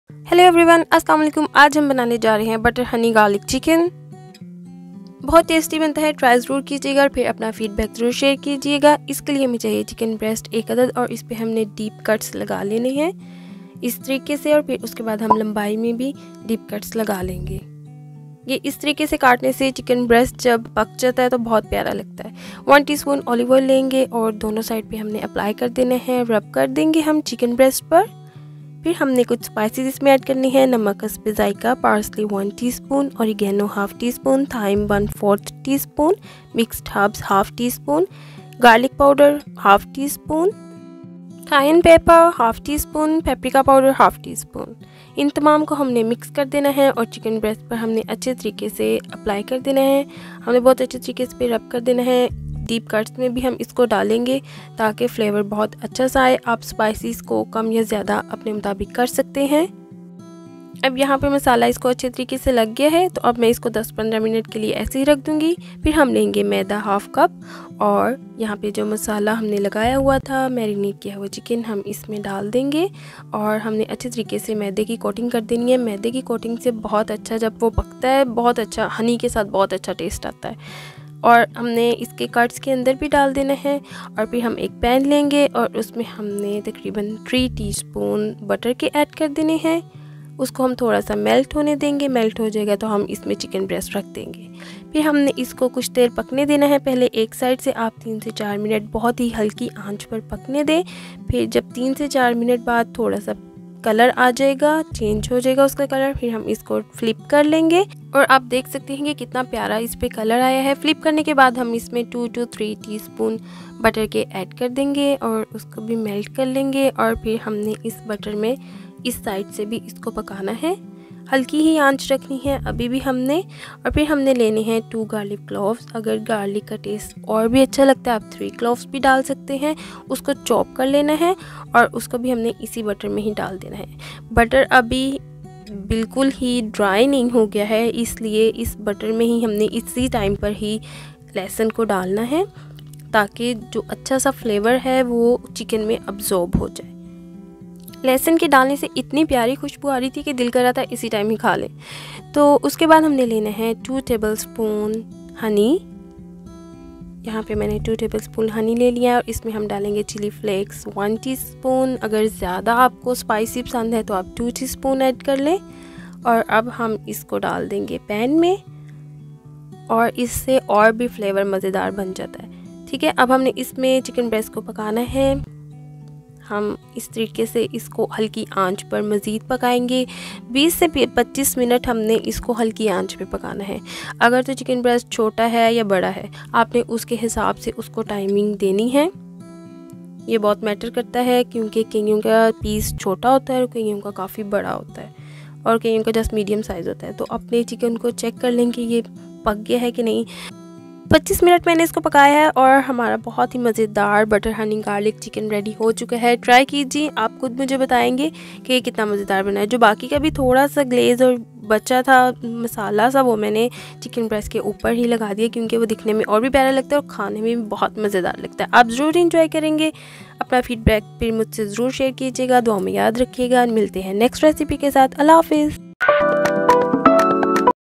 Butter Honey Garlic Chicken..An Amazing Recipe 👏
I'm sure you will love it..it's not like typical dry chicken.its full of flavour & aroma..try enjoy, thanks me later and share your feedback.